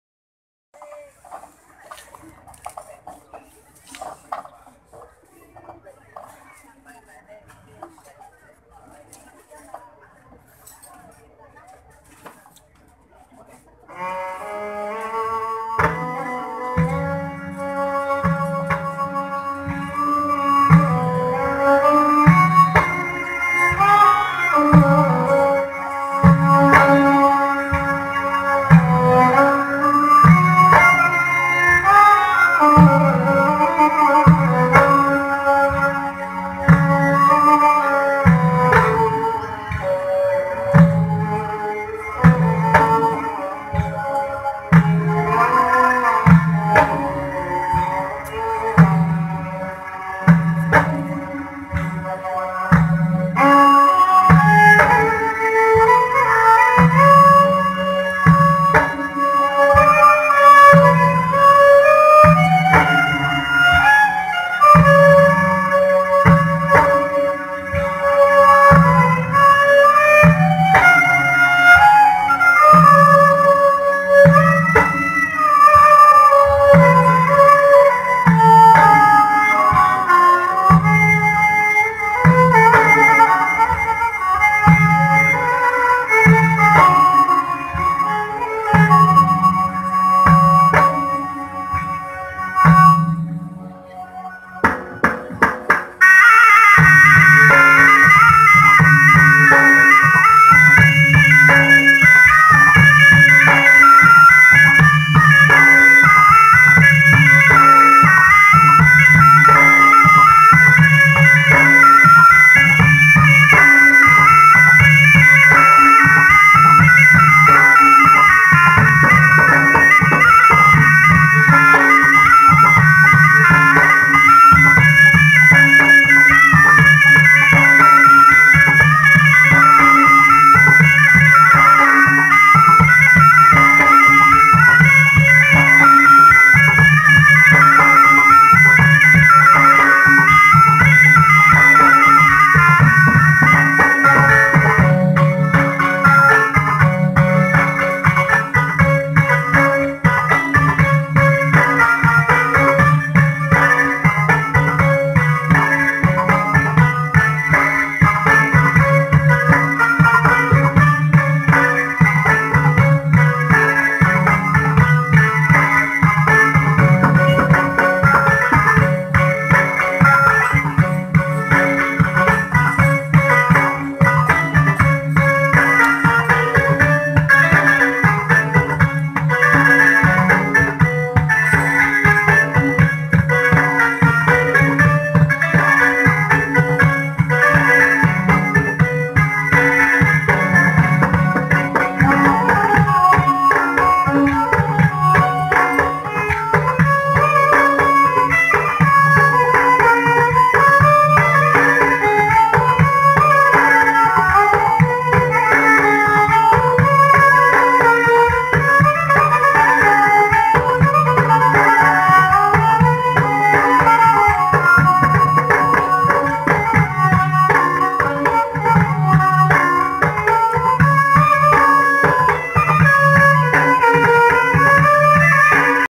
Tari Ranup Lampuan pada Wisuda SMKN Taman Fajar Angkatan X
Tarian ini disajikan pada Wisuda SMKN Taman Fajar Angkatan X Tahun 2019